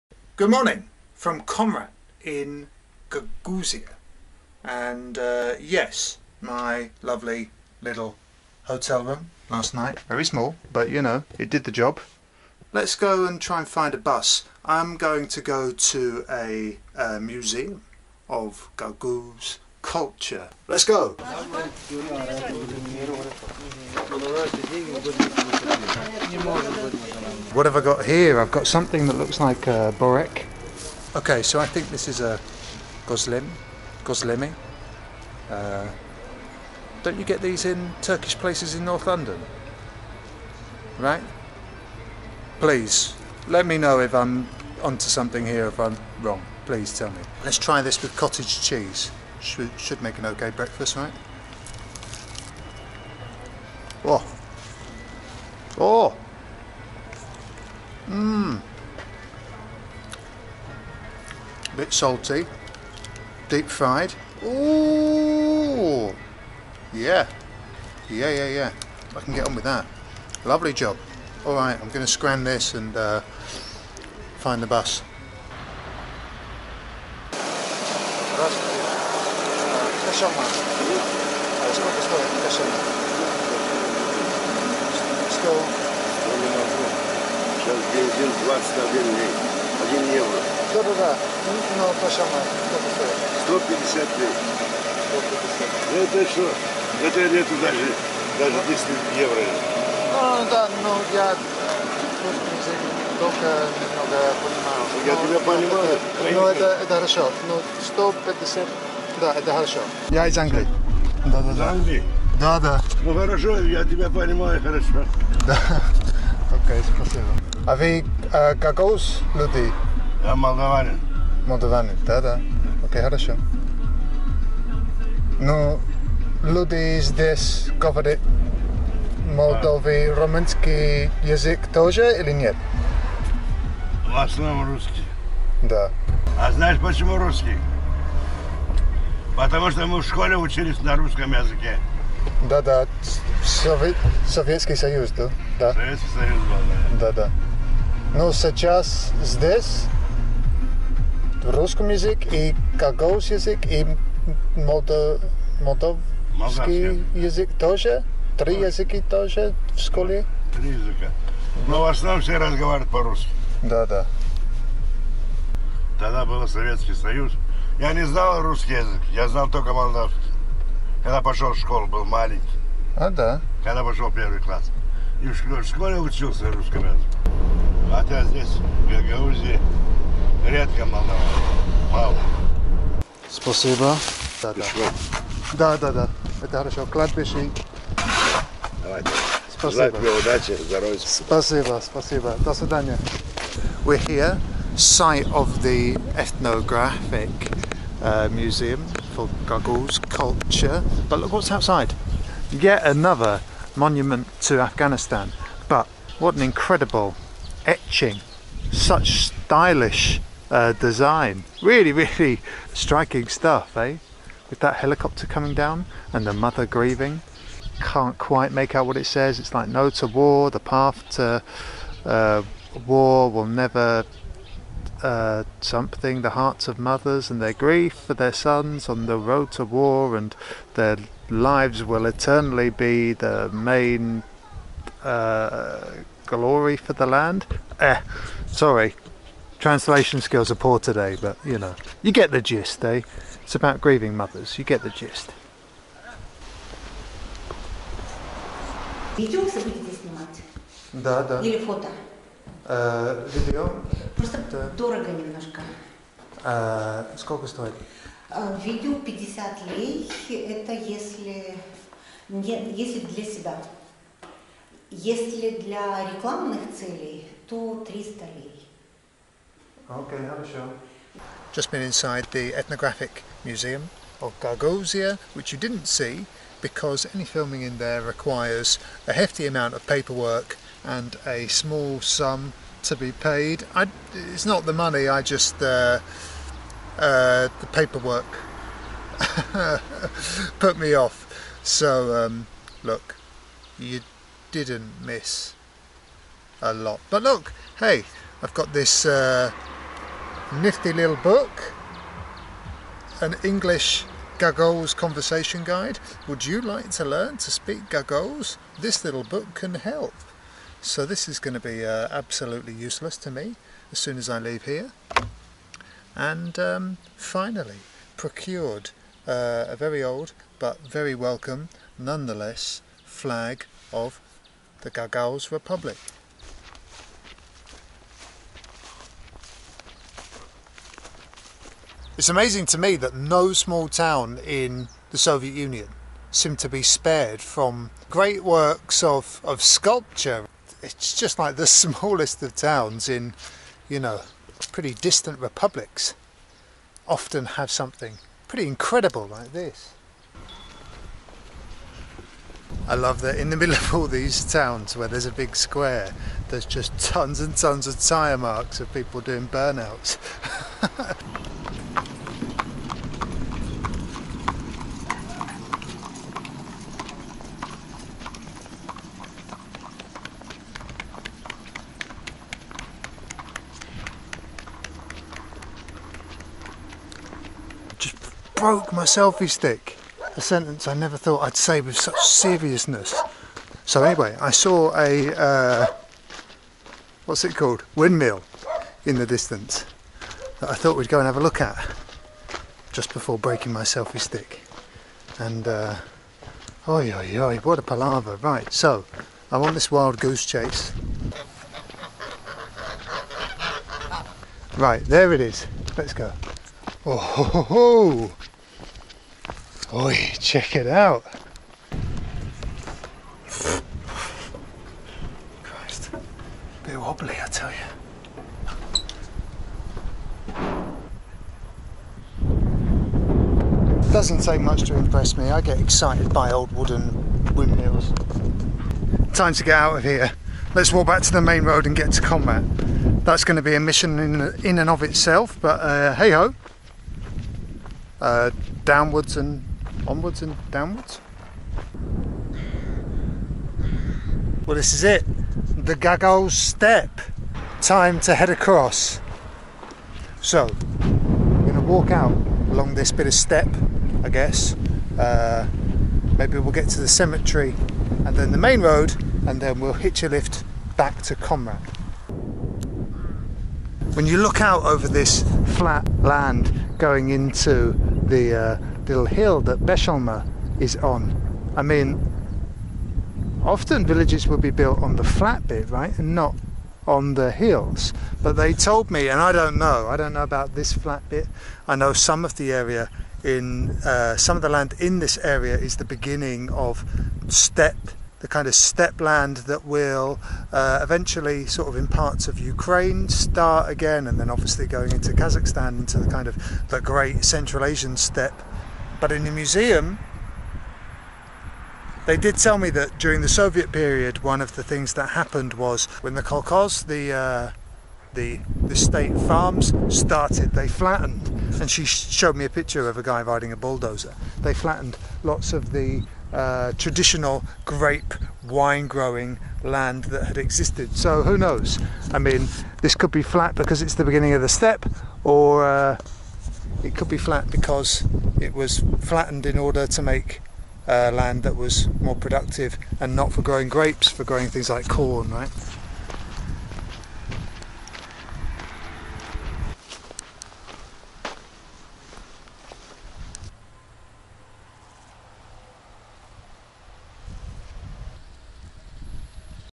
The Christian Turk villages of the European steppe
In the final video from Gagauzia we cross the steppe to find yet more Soviet monuments. Along the way we meet more people who call this great part of Europe home.